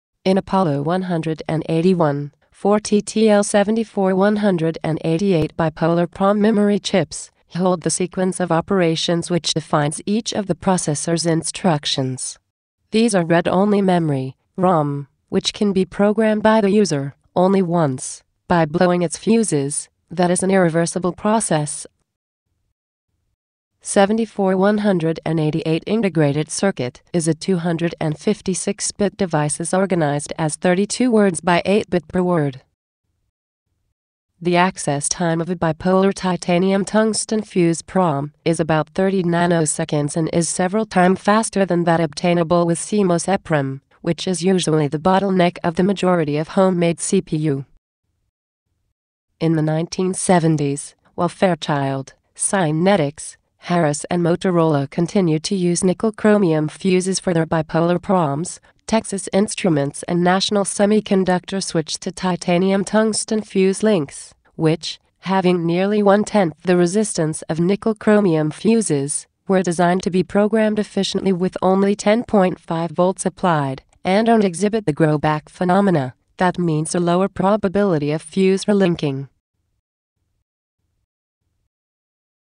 Let's program a ROM (Read Only Memory): the TTL 74188 PROM (APOLLO181 Construction)
APOLLO181 Project G.G. 2012 Copyright

Technology:
TTL Schottky (1974 Bugbook® era technology)

On-board RAM:
256 byte

On-board ROM:
64x16 bit

ALU type:
74181 (SN74S181)

Data word size:
4-bit

Instruction word size:
8-bit

Clock (max):
3MHz

Processor speed (max):
0,75 MIPS

Number of basic instruction:
16 plus 32 arithmetic and logic function

Number of ICs:
60+